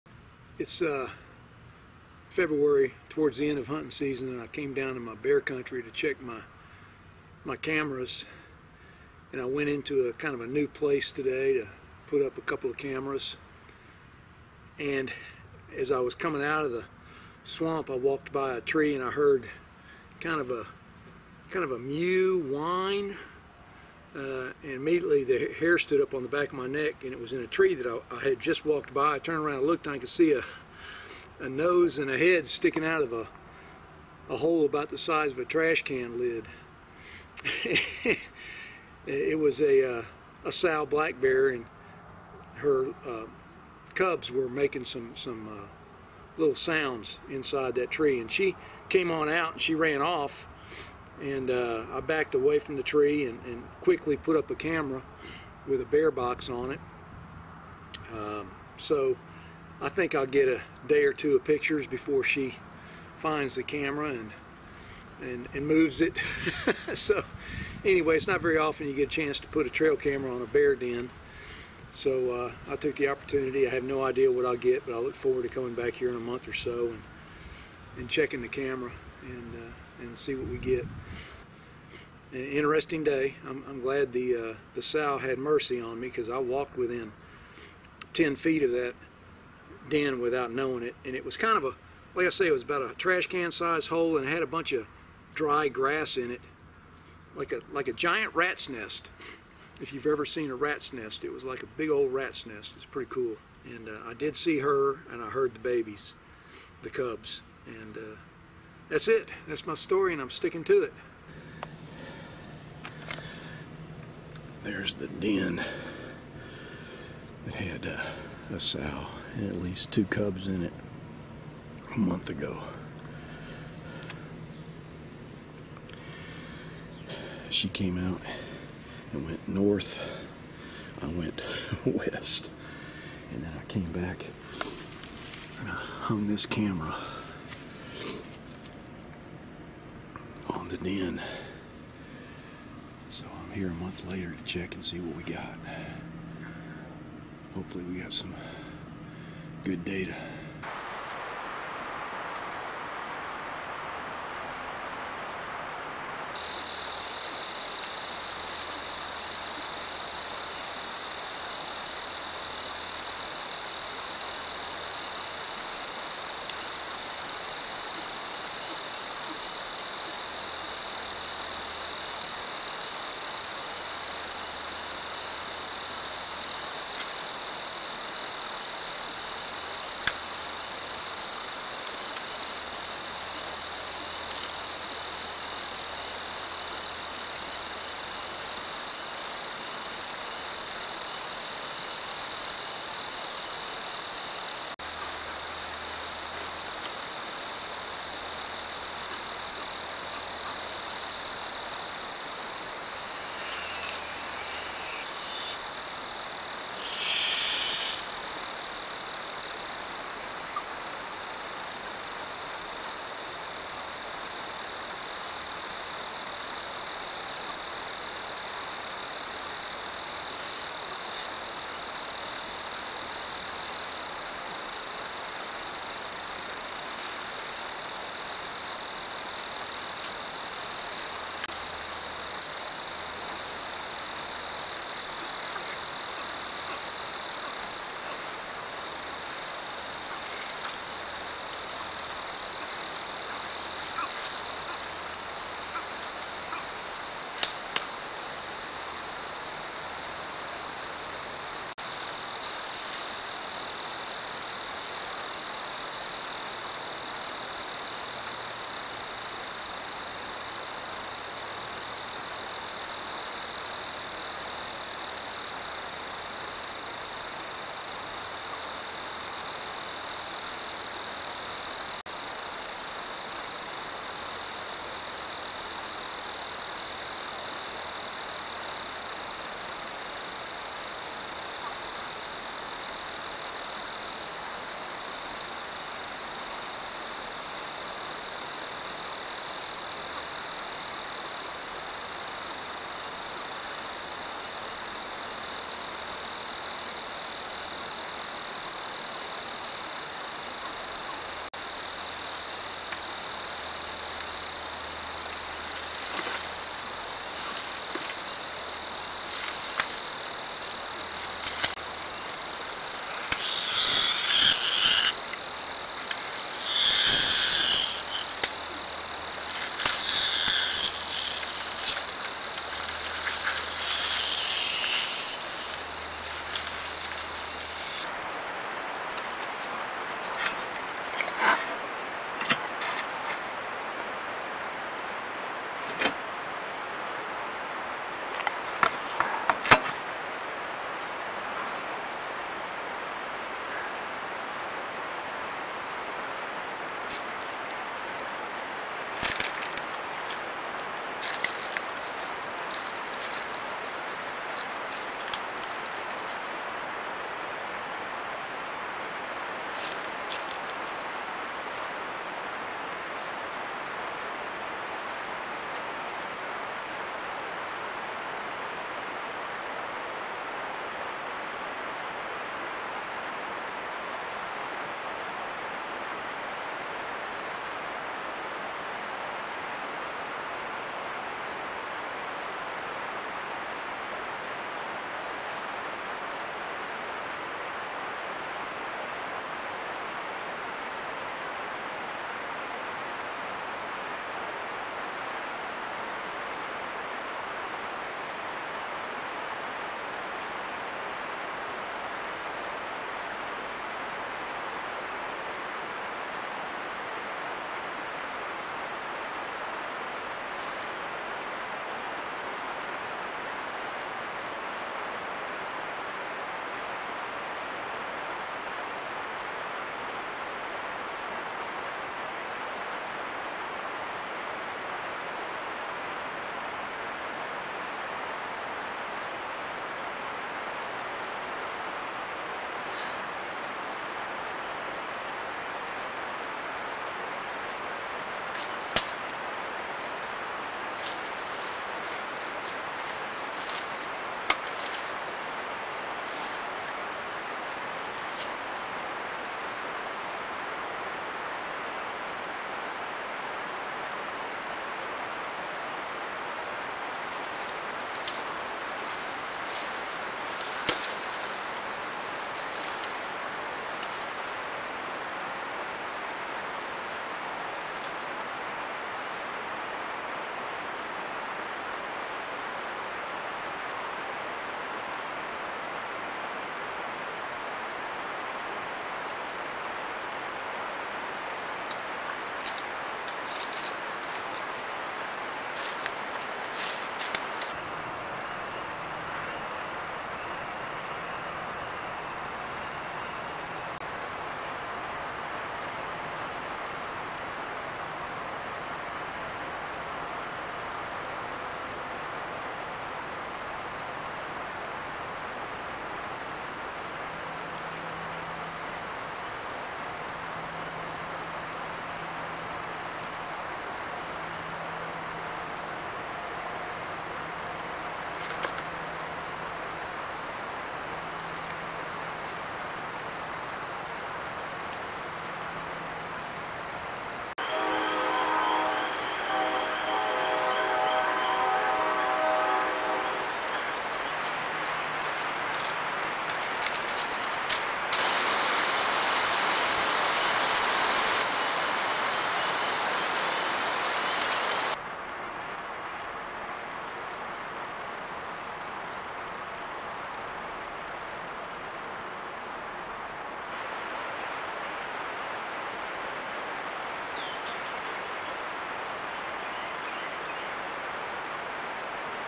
Swamp Cam Video, BLACK BEAR DEN, South Louisiana
I located the winter den of a sow black bear and cubs. I had no idea that within 8 hours of my intrusion the sow would relocate the cubs to another area. I regret putting the camera on the den because it disturbed the mom and her cubs. I'll know better next time.